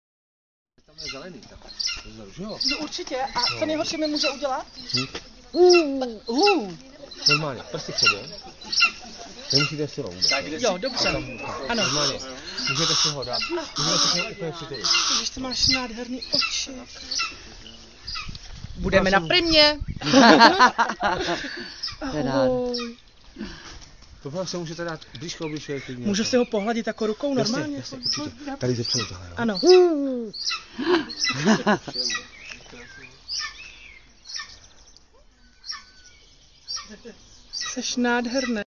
Já a dravec
Výr František, 19 let, zámek Sychrov - Autor: Kateřina Jirušová